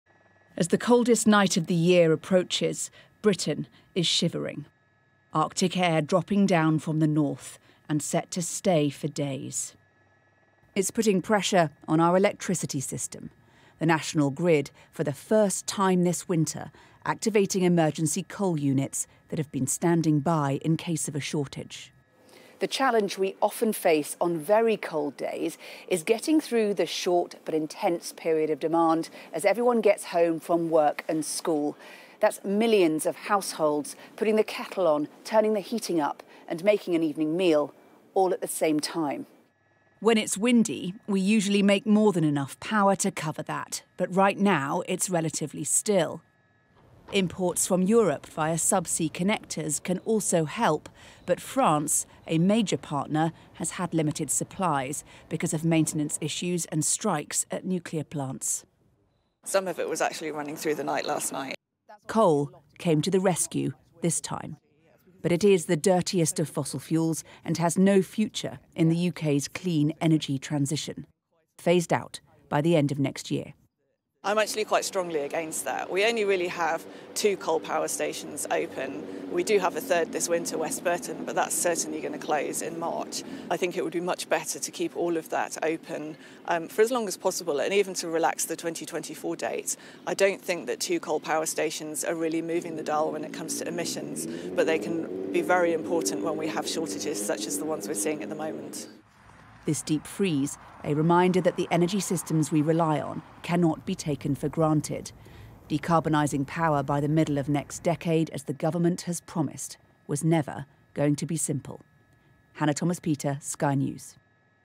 Emergency coal power used for Britain's coldest night of the year
For the first time ever, two of five emergency coal-fired generators will be required to produce electricity to help prevent the lights from going out in Britain.

Temperatures overnight are forecast to dip around -15C in parts of Scotland due to arctic air dropping down from the north.